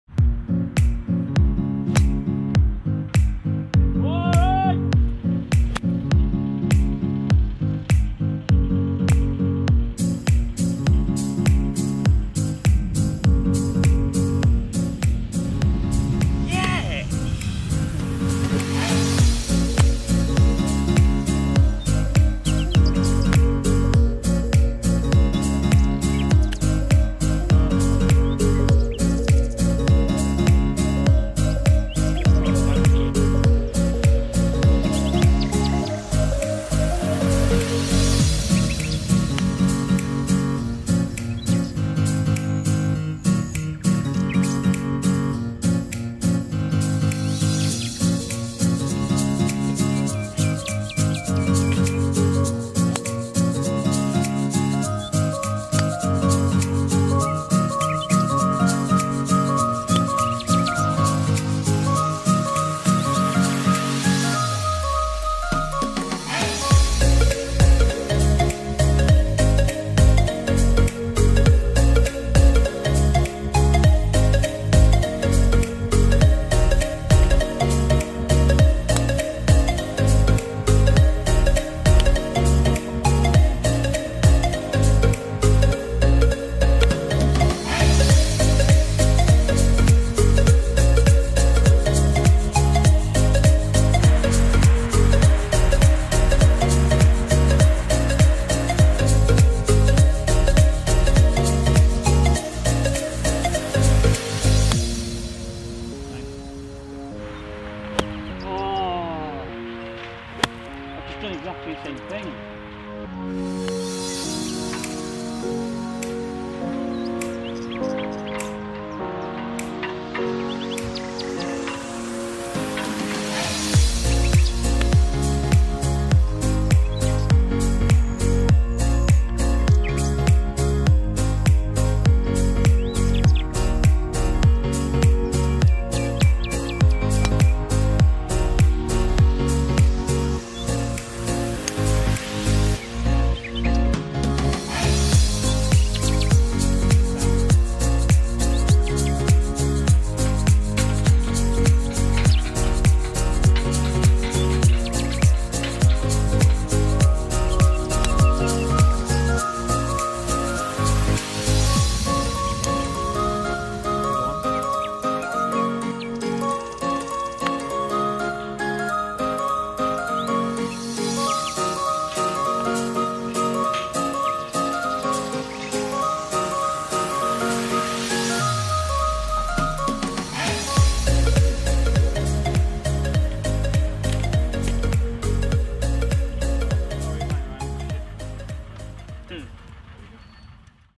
Every Shot of a 30 Handicap Golfer's Round
Here is every shot of a 30 handicap golfer's round, but believe me when I tell you he definitely should be coming down a fair few shots. I play with him after all!